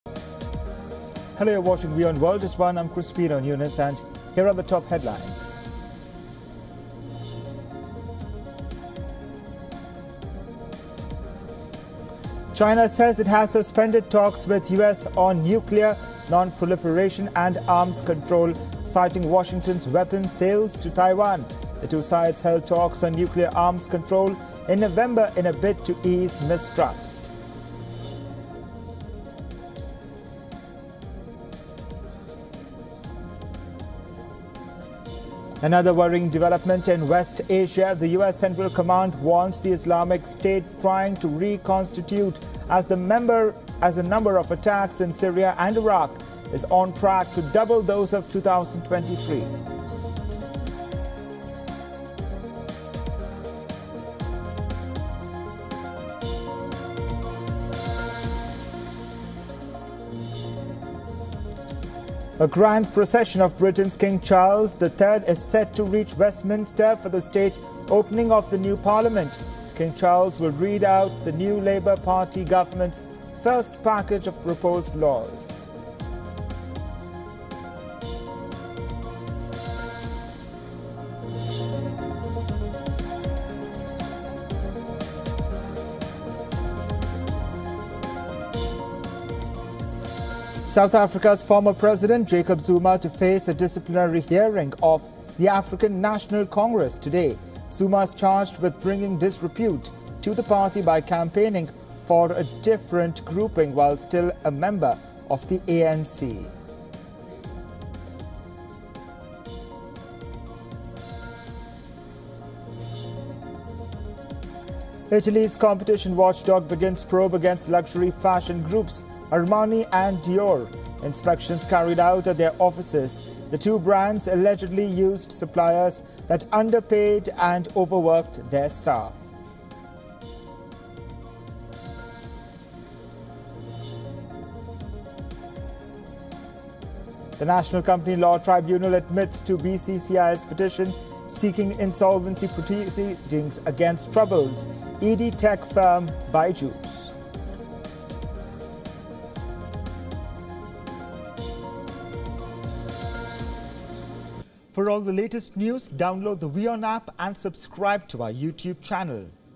China suspends nuclear arms control talks with US| I-S attacks in Iraq & Syria spike| WION Headlines
Headlines of the hour:

- China suspends nuclear arms control talks with US
- I-S attacks in Iraq & Syria spike
- King Charles III to outline Starmer govt's to-do list
- South Africa: Zuma to face ANC disciplinary hearing